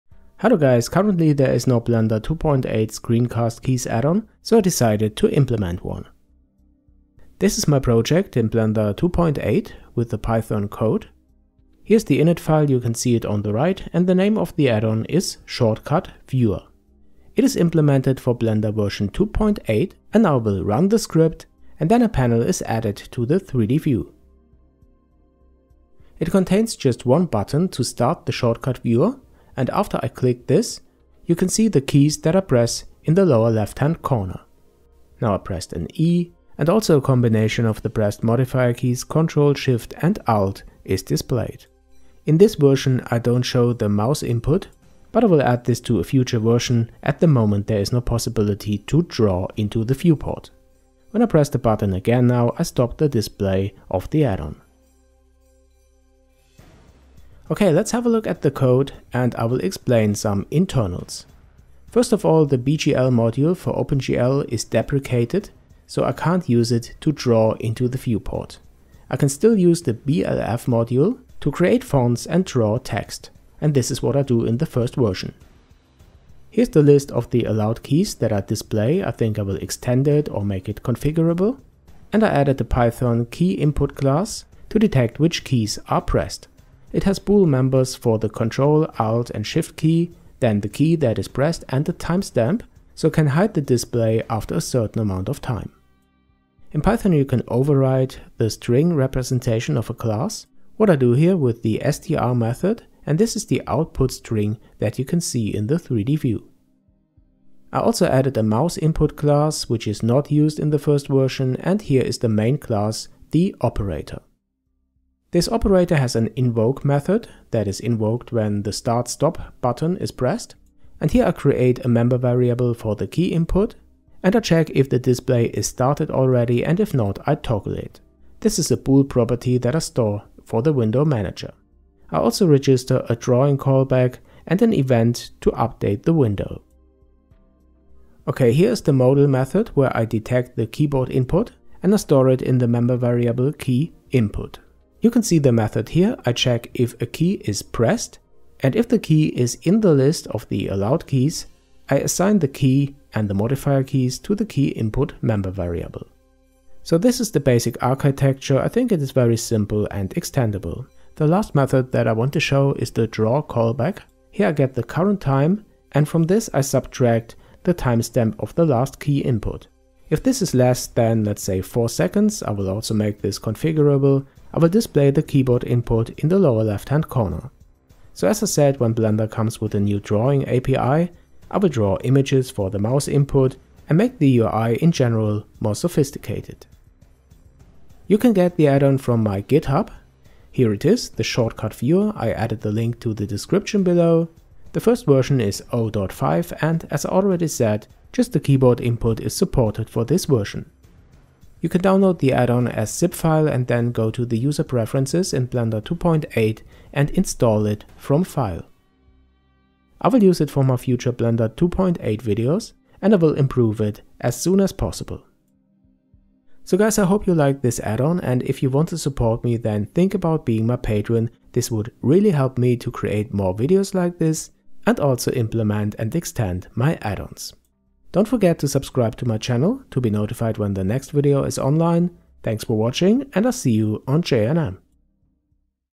Blender 2.8 Screencast Keys Addon
In created a new Blender 2.8 screencast keys addon to display Blender keyboard shortcuts.

Since BGL, the opengl module is deprecated and not supported for Blender 2.8 at the time being there is no mouse input display, but I will add it in a future version when Blender comes with a drawing API.